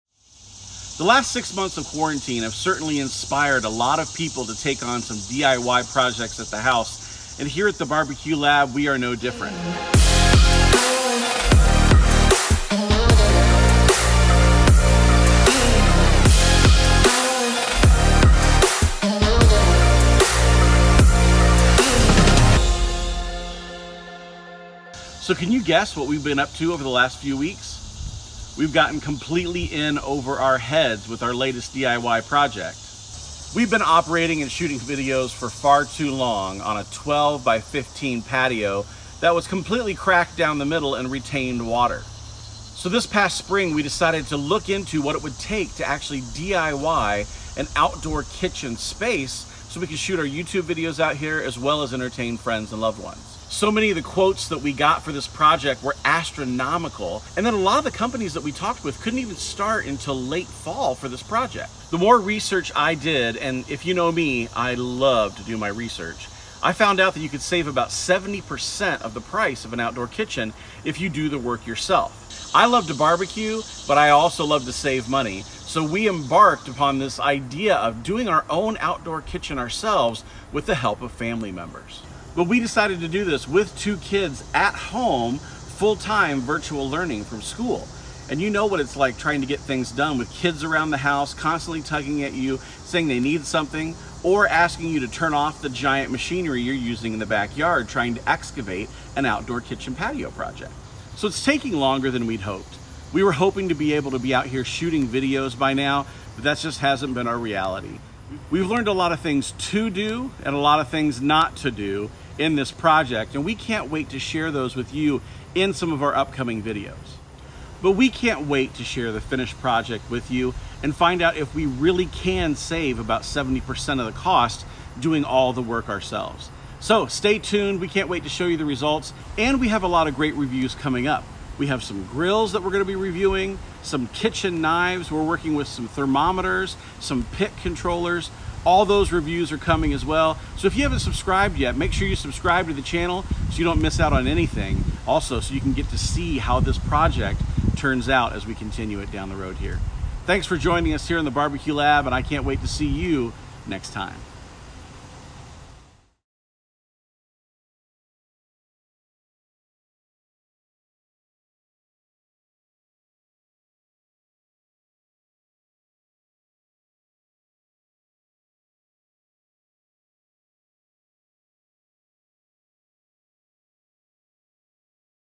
DIY Outdoor Kitchen Project Announcement! | How to DIY an Outdoor Kitchen
With the arrival of this year's stay-at-home mantra, we're learning how to DIY an outdoor kitchen! We've been operating for so long on a broken down 12x15 patio, and it's just not big enough to shoot YouTube videos, house 2 grills, a fire pit, and seating for a family of  4.

So, we've embarked on a months-long journey of building a much larger patio, outdoor kitchen and covering the whole thing with a pavilion. Turns out it's a lot of work, but we're excited to see the end result!